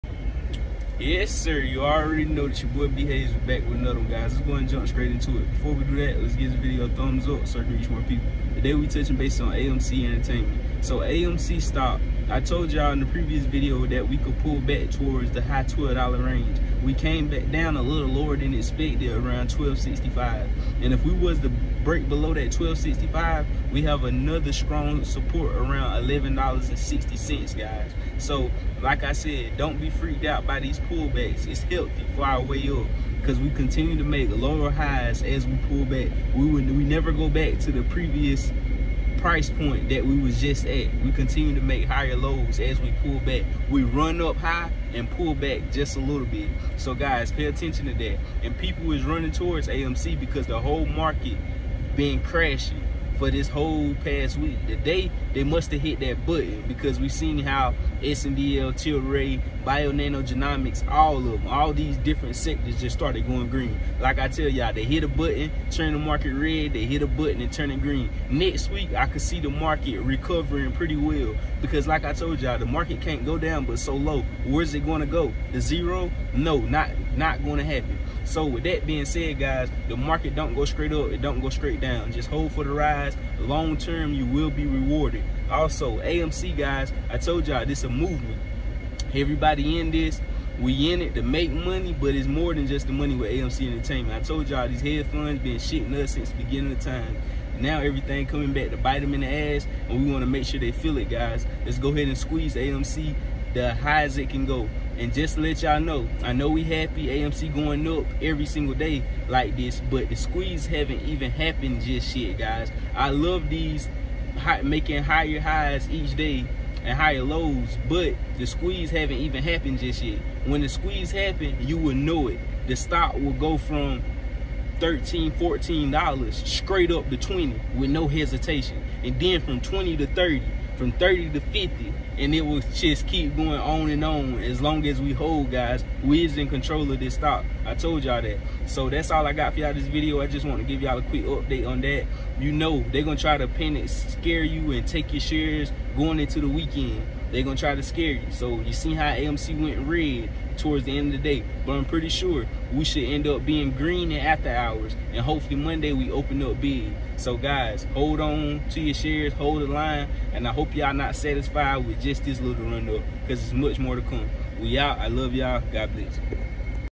🔥AMC LONGEST WIN STREAK IN 2 YEARS
In this video we will cover amc & expectations for tomorrow. AMC support & resistance technical analysis.